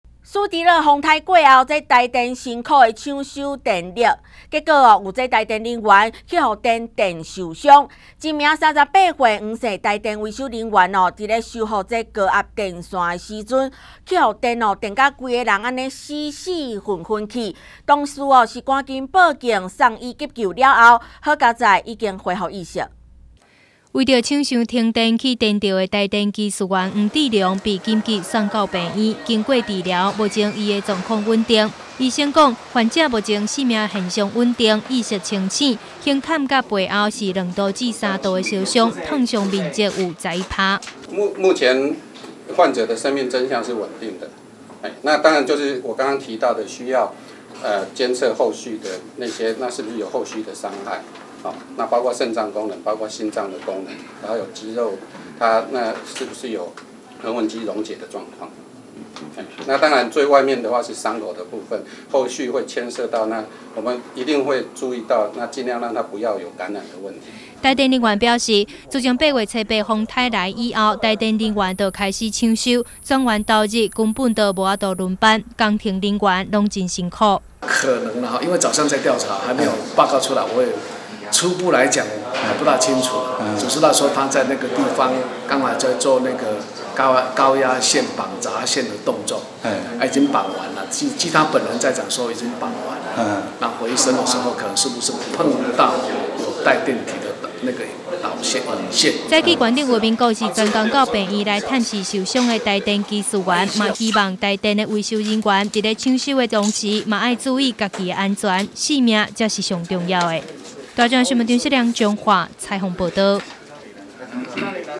1040812搶修停電 台電技術員遭電擊送醫
蘇迪勒颱風來襲造成全台大停電，彰化縣的停電狀況最為嚴重，台電工程人員頂著烈日搶修，不料卻發生電擊意外，在緊急送醫後生命跡象穩定，意識清醒，前胸後背2到3度灼傷，燙傷面積11％。